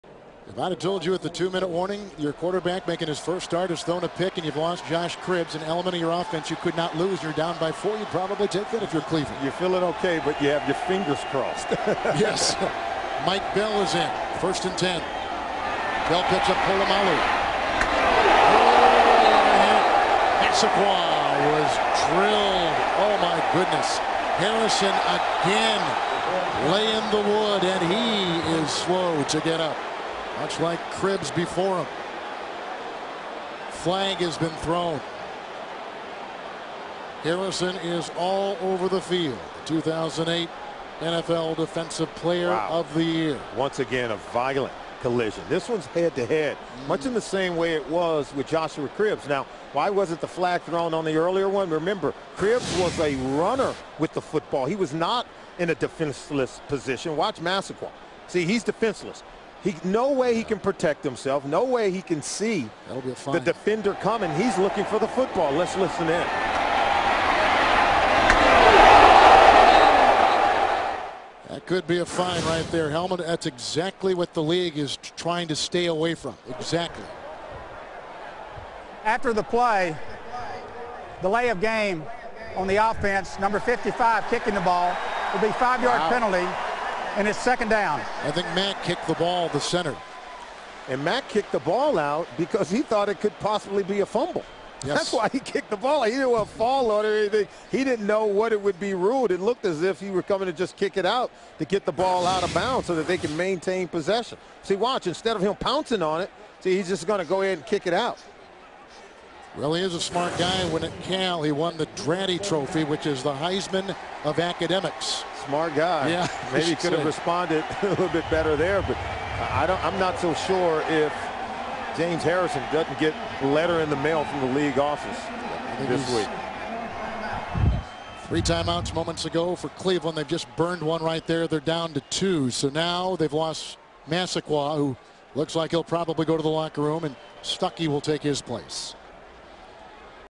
James Harrison Hit on Mohamed Massaquoi
James Harrison’s brutal hit on Mohamed Massaquoi from October 17, 2010. Browns vs. Steelers. Harrison was fined $75,000 for this hit as a repeat offender for Harrison’s hit on Massaquoi. Recently, Harrison has said that Mike Tomlin gave him an envelope after the game. Since then, Harrison has assured it was not a bounty, but we may never know.